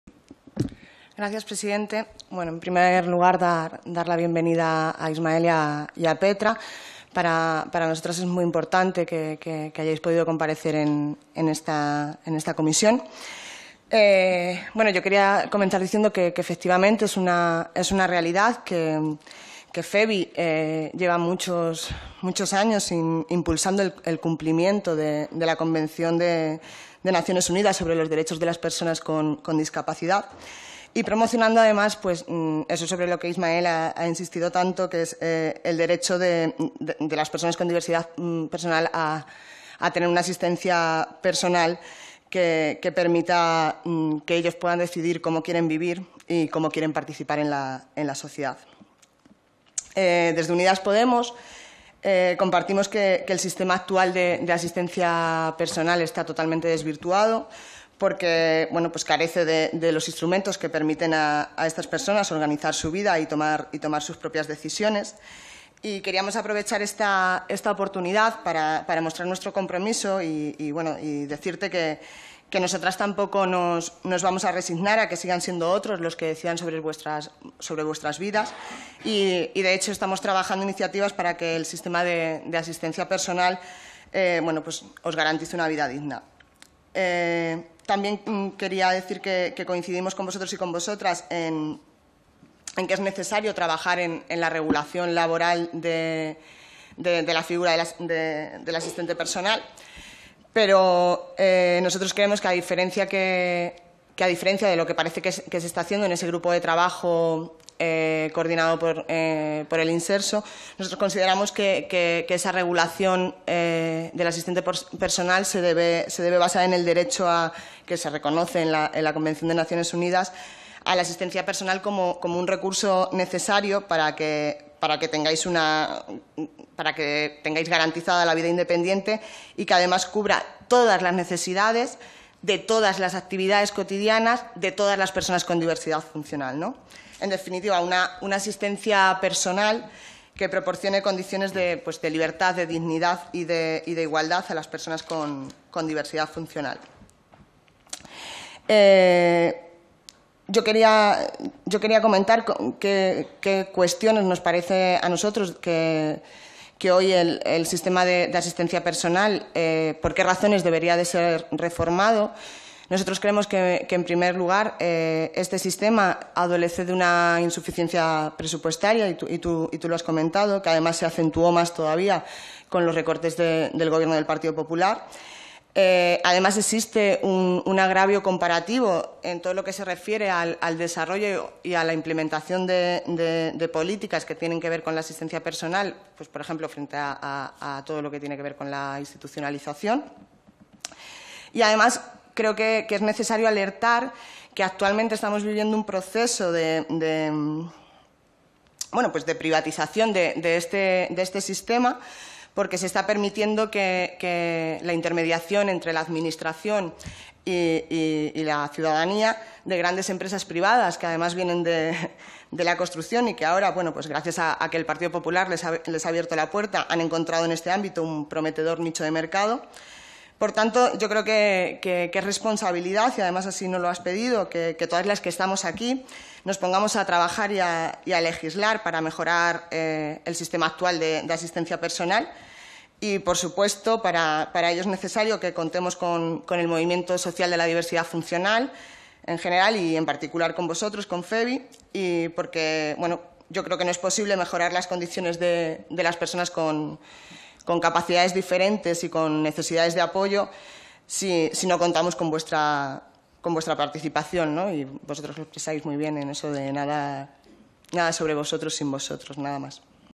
Teresa Arévalo en la Comisión para las Políticas Integrales de la Discapacidad. 20 de febrero
Durante la comparecencia de Ismael Llorens Santamaría, Presidente de la Federación de Vida Independiente (FEVI), para informar y trasladar propuestas y demandas sobre vida independiente y asistencia personal.